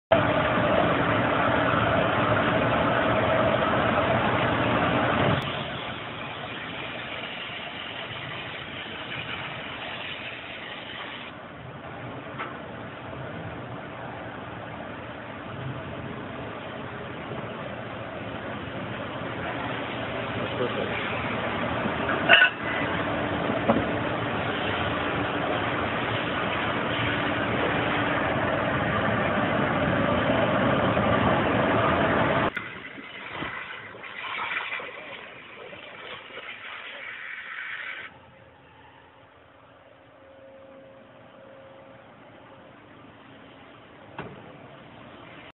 2011 Flow Mach 2 3120b 6.5' x 10' CNC Waterjet Ref # 8068709
2011 Flow Mach 2 3120b 6.5' x 10' CNC Waterjet Ref # 8068709

SPECIFICATIONS
Table Width 6.5'
Table Length 10'
Control Flowmaster 6.7
Pump 30 HP
Max Cutting Pressure 60,000 PSI
Power Requirement 480/3/60
Linear Straightness .005"/3'
Rapid traverse speed 400" Per Minute
Repeatability .0025"

EQUIPPED WITH:
Cutting Head
Flowmaster CAD Software
Flownest Part Nesting Software
Paser ECL
Abrasive Hopper
All Manuals
Under Power